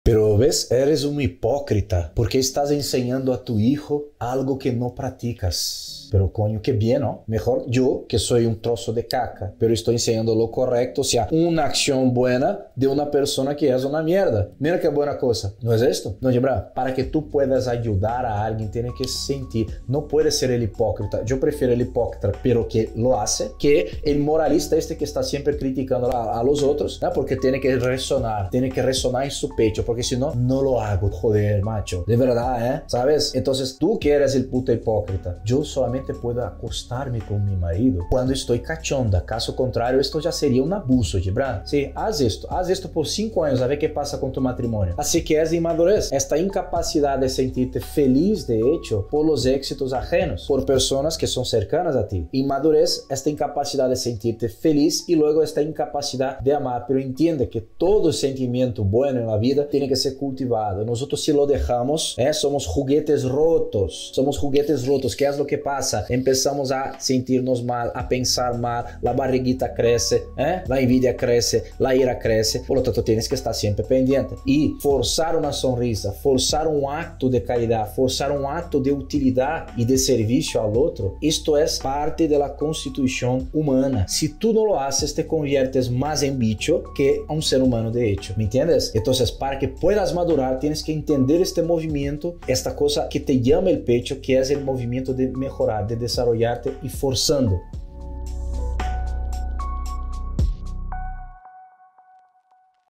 📢 ¡El amor no es algo que te pica el pecho!🤯
Es hora de enfrentar la realidad y dejar de ser hipócrita. ¿Te atreves? Aprende cómo alcanzar la madurez en todas las áreas de tu vida, de manera personal y transformadora.👉 La incapacidad de celebrar el éxito ajeno, la envidia que crece y la ira que nos consume, todo esto es inmadurez. Pero tranquilo, hay una solución. ¡Yo te mostraré el camino!💛 Atrévete a sentir, a amar, a mejorar, a desarrollarte. Forzar una sonrisa, un acto de caridad o de servicio es parte de nuestra esencia humana. ¡Así es como maduramos de verdad!✅ ¿Listo para dar el paso hacia una vida más madura y significativa? ¡Sígueme y descubre cómo lograrlo!GibraanHanna